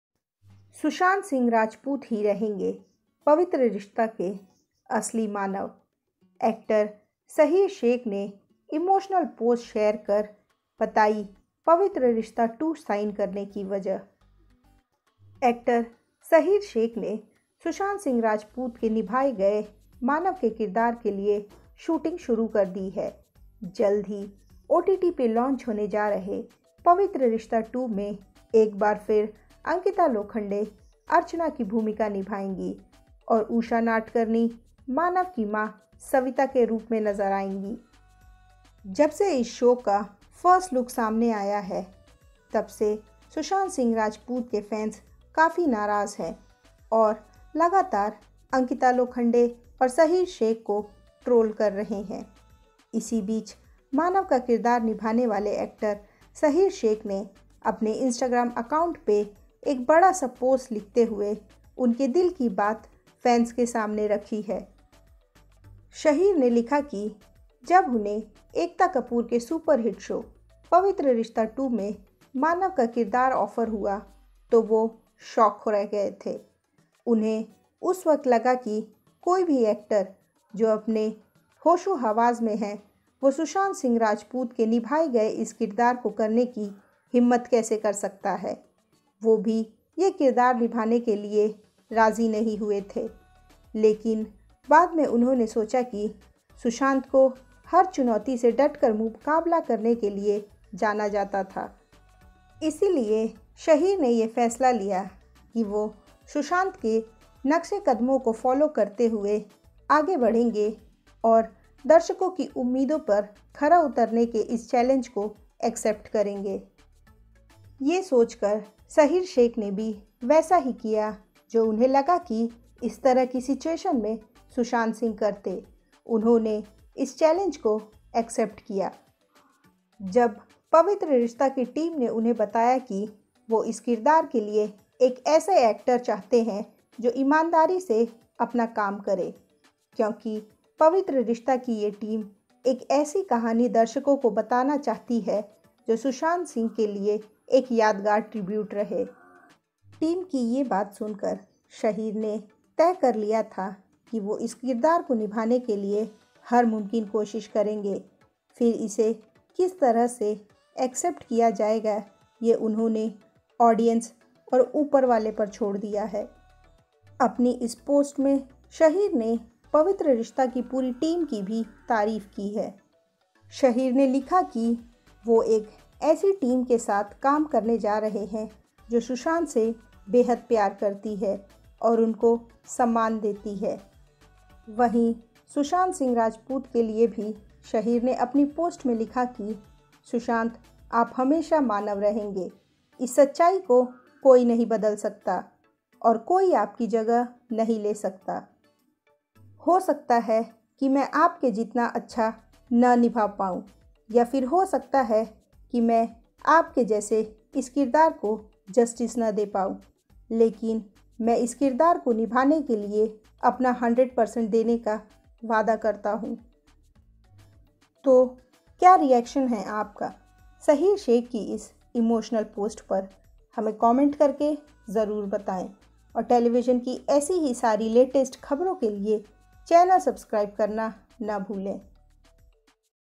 Shaheer Sheikh Reveals WHY He Agreed To Do Pavitra Rishta 2, says "Scary To Step Into SSR's Shoes"
Pavitra Rishta 2.0: Kuch Rang Pyar Ke Aise Bhi Actor Shaheer Sheikh Reveals Why He Agreed To Play Manav In Pavitra Rishta 2, says "It's Scary To Step Into Sushant Singh Rajput's Shoes"